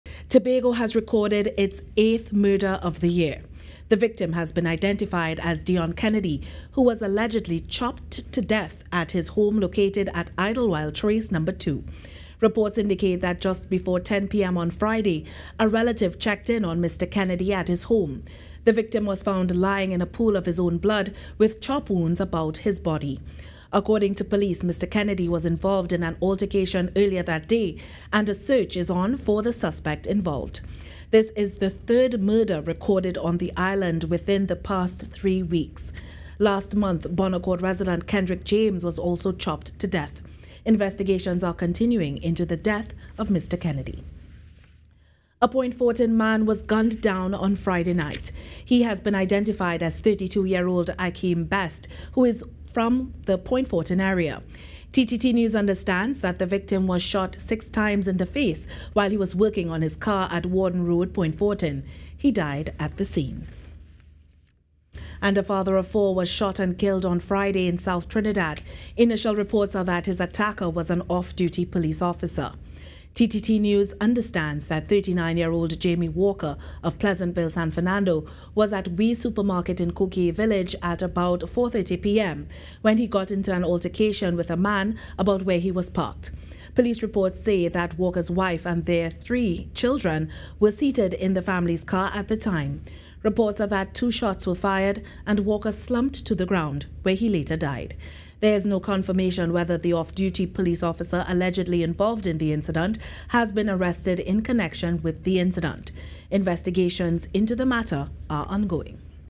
Crime Round-Up: Three Men Killed In Separate Incidents
Tobago has recorded its eighth murder for the year.

The victim has been identified as Deon Kennedy, who was allegedly chopped to death at his home located at Idle Wild Trace #2. Reports indicate that just before 10pm on Friday, a relative checked in on Mr. Kennedy at his home. The victim was found lying in a pool of blood with chop wounds about his body. According to police, Mr. Kennedy was involved in an altercation earlier that day, and a search is on for the suspect involved.

Elsewhere, a Point Fortin man was gunned down on Friday night. He has been identified as 32 year old Ikem Best. TTT News understands that the victim was shot six times in the face while he was working on his car at Warden Road, Point Fortin. He died at the scene.

And a father of four was shot and killed on Friday in South Trinidad. Initial reports are that his attacker was an off duty police officer.

TTT News understands that 39 year old Jamie Walker of Pleasantville, San Fernando was at WE Supermarket in Cocoyea Village at about 4:30 pm, when he got into an altercation with a man about where he was parked. Police reports say that Walker's wife and three of their children were seated in the family's car at the time.

Reports are that two shots were fired and Walker slumped to the ground, where he later died. There is no confirmation whether the off duty police officer allegedly involved in the incident, has been arrested in connection with the incident.

Investigations into the matter are ongoing.